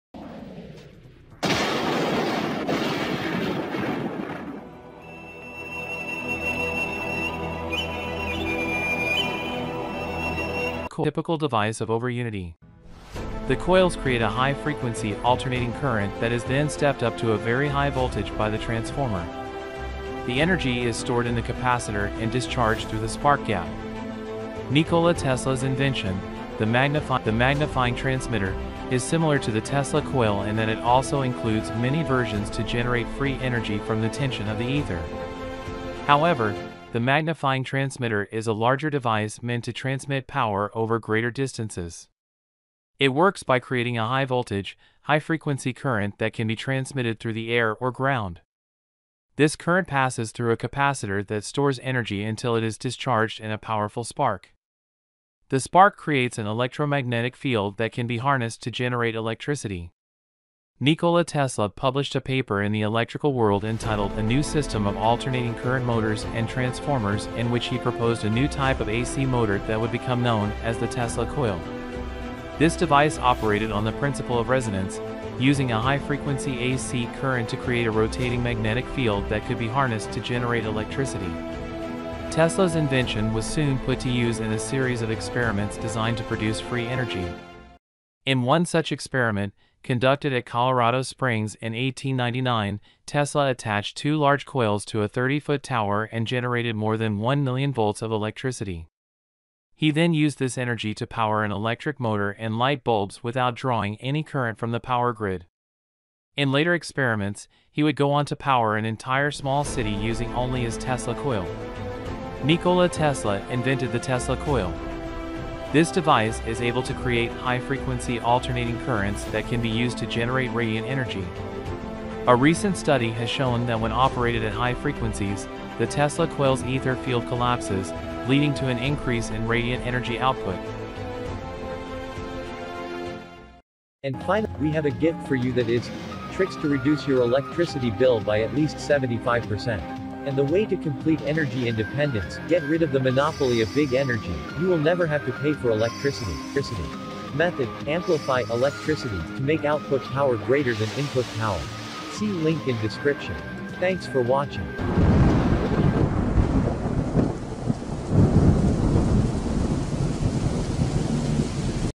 Ultimate Energizer Guide Review - Tesla Coil Version Generates Free Energy
Free Energy From Tesla Coil - Tesla Transformer Become Generator - by Ultimate Energizer Guide!

The Tesla coil has been around for over a hundred years and was created by Nikola Tesla. It is said that he developed it to provide free energy, and although it's never been proven, the Tesla coil remains one of the most impressive pieces of technology ever invented.

There are many different versions of the Tesla coil, but the most popular is the classic spark gap design. This version creates a high-voltage, low-current electric spark that can be used to power devices or create light displays.

The Tesla coil is also known as an "ultimate energizer" because it can be used to power other devices such as electric motors, fluorescent lights, and even lasers. In fact, some people have even built their own mini Tesla coils using kits that are available online.